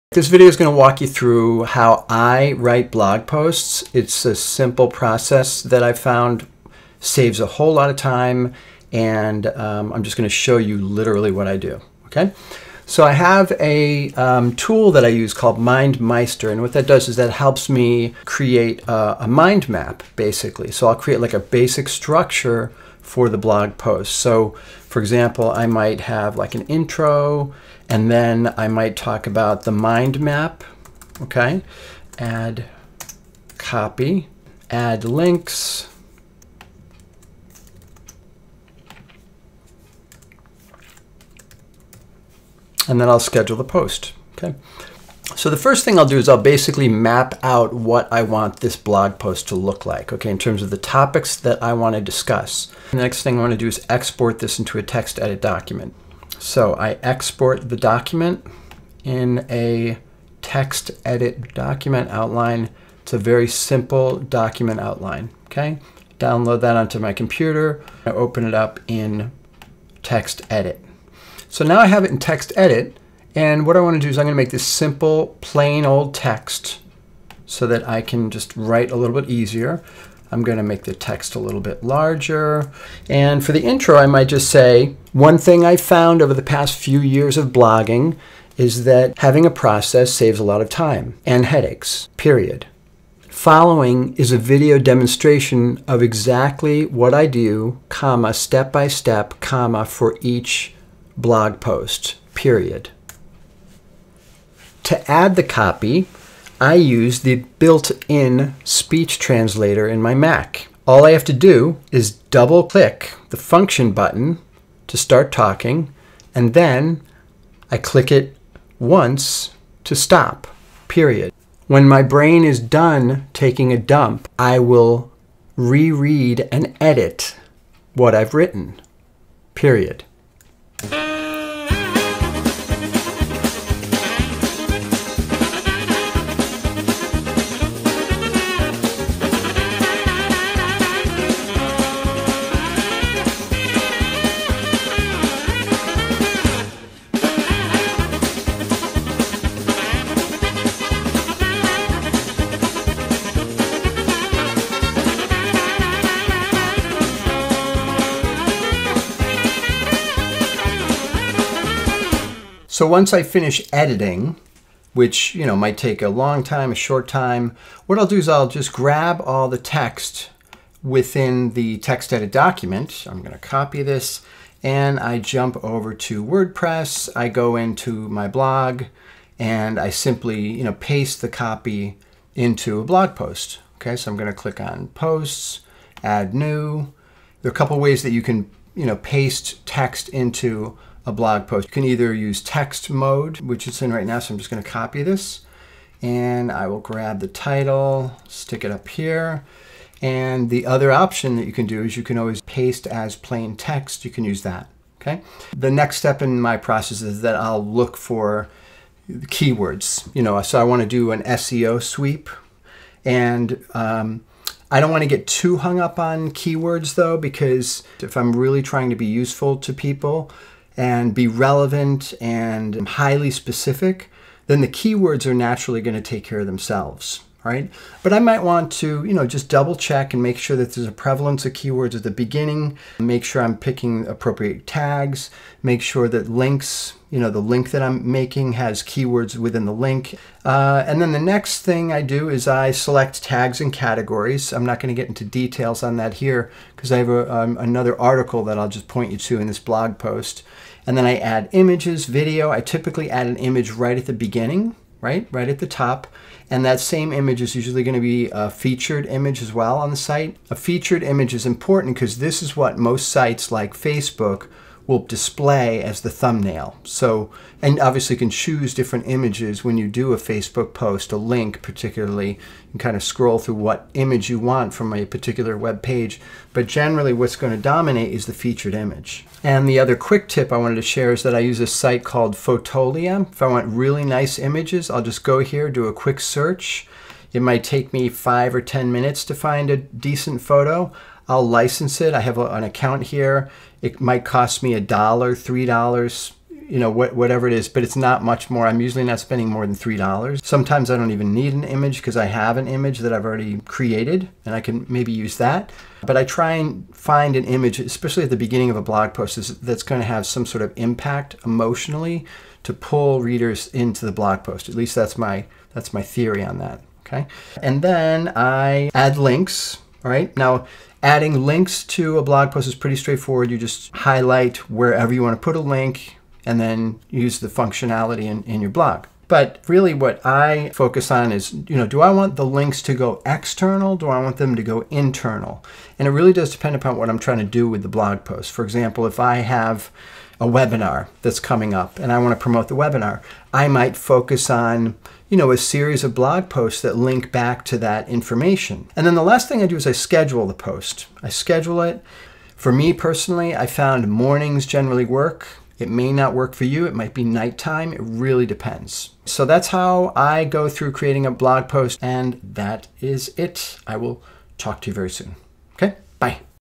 Time-Saving Hacks to Write More Blog Posts - A Video Demonstration
Here's a 6-minute video demonstration of exactly what I do, step-by-step, for each blog post:

Make an outline - Assuming you've selected useful topic to write about, all you need at the beginning is a basic framework to support the copy.

Dictate Copy - I use the built in speech translator in my Macbook Pro to write copy. Begin with a brain dump, and then edit what you've written.

Edit the Copy - Eliminate as much copy as possible without eliminating your voice. Write like you speak, but keep it short and sweet.

Transfer the Copy - Copy the content from your plain text editor, and copy it in your blogging software.

Tweak SEO - It's my belief that when you write content that's highly specific and useful to your audience, the SEO takes care of itself.

Add Tags and Categories - Next, select the appropriate categories and tags for your blog post. Categories should represent the larger topics within your blog, and tags should represent specific elements that are within various categories.

Add Images - Images are honey, your readers are bees. They should trigger readers on an emotional level to stick around and read more.

Add links - Depending upon your goals, you might link to internal pages, or link to external pages. For example if you're trying to promote an event, you might write a series of blog posts linking to the registration page.

Schedule Post - Finally, schedule the blog post for a morning within the next day or two (bonus points if you know what time is best for your community).